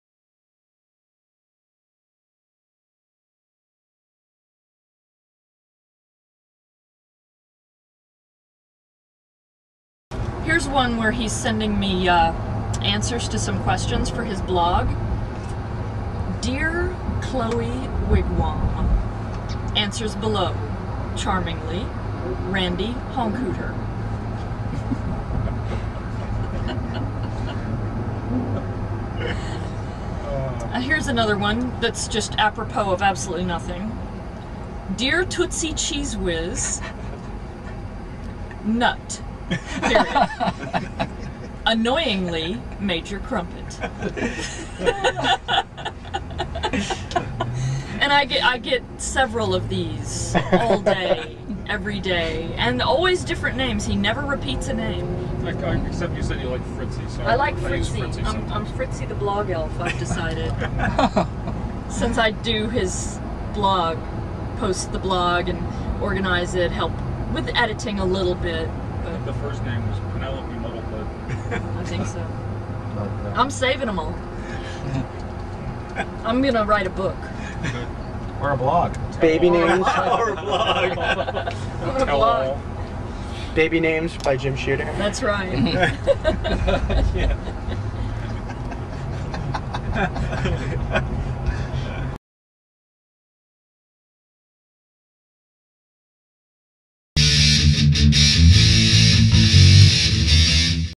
Baby Names by Jim Shooter
Jay Jay Jackson talks about some of the clever names Jim Shooter comes up in everyday correspondence.